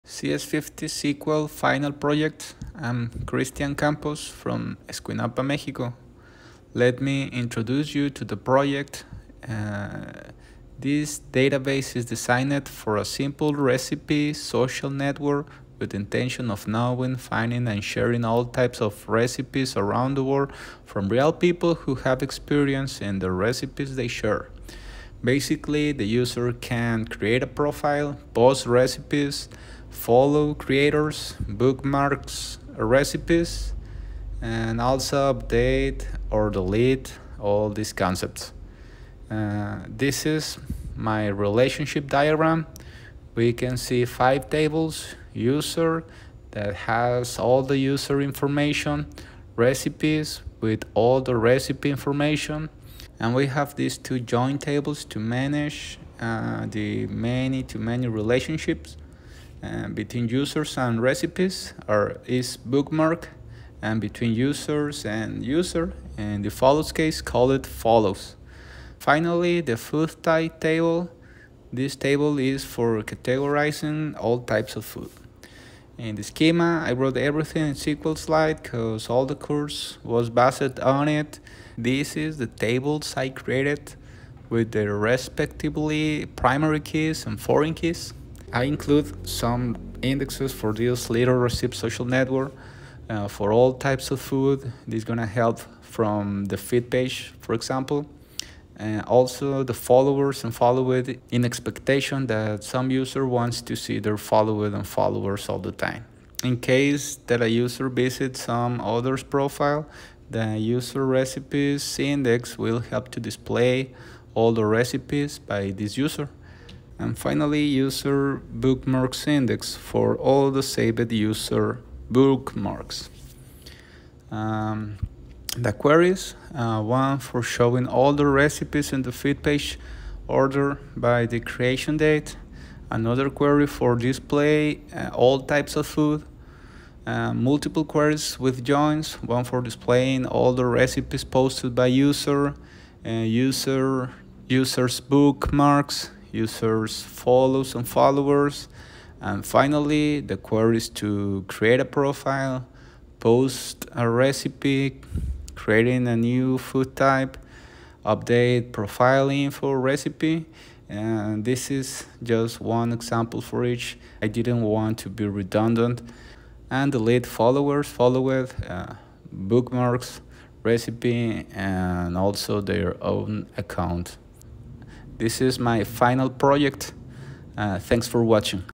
CS50 SQL Final Project | Recipe Social Network | 2024
Attending to the final project document, I present the creation of the database for a simple social network of recipes.